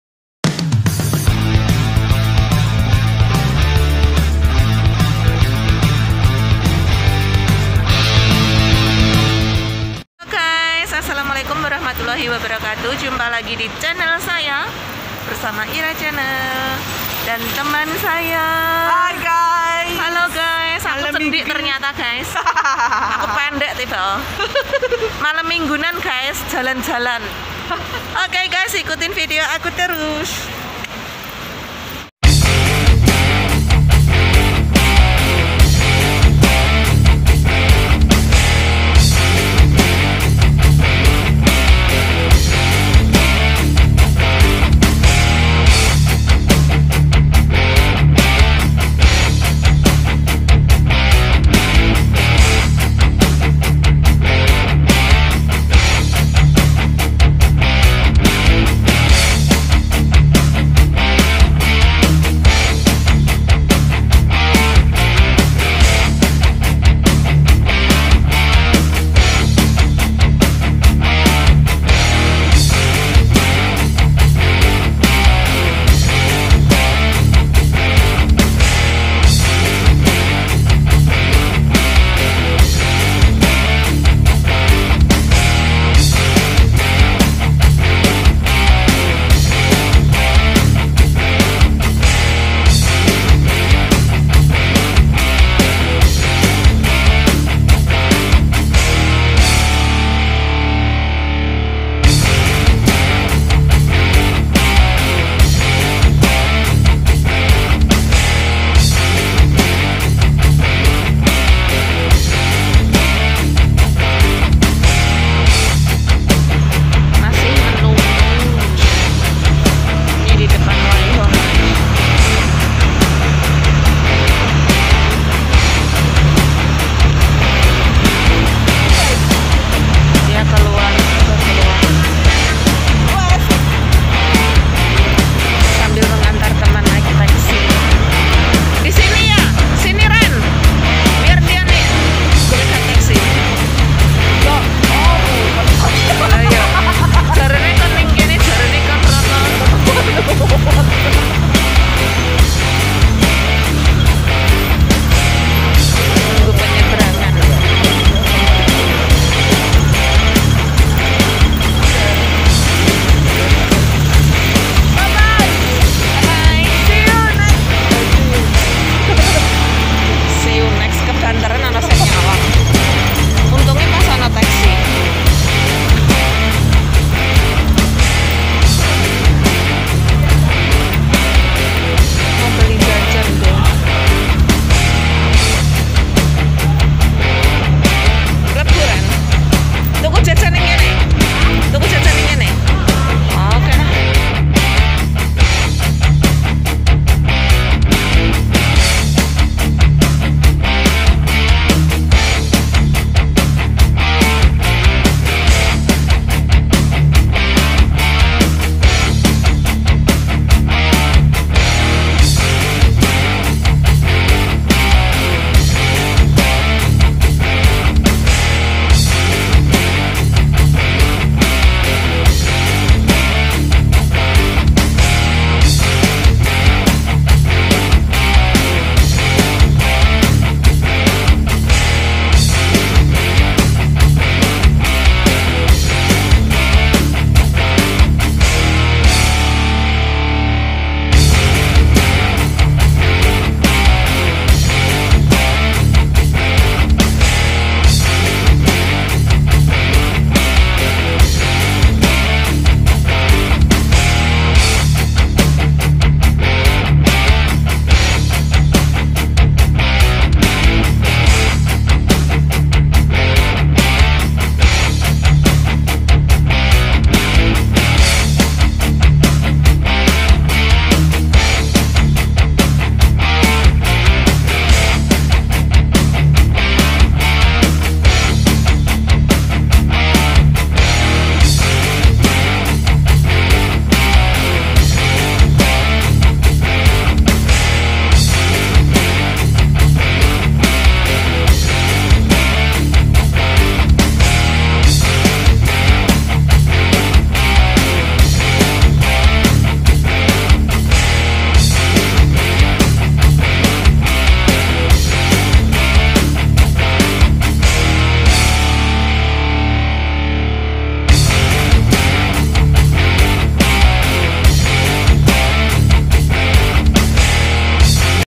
KERAMAIAN MALAM MINGGU DI MONGKOK
Dipublikasikan tanggal 7 januari 2022

Assalamualaikum
 hallo guys

Video kali ini saya berada di area mongkok city
Jalan jalan malam minggu menikmati angin malam dan keindahan kota mongkok di saat malam tiba
Di tempat ini banyak sekali tempat tempat yang Bagus dan menarik
Seperti mall rumah makan ataupun toko Indonesia yang menyediakan makanan Indonesia tentunya
Ladies market, rumah makan, pasar tradisional, langham place, dan masih banyak lagi tembat hiburan lainnya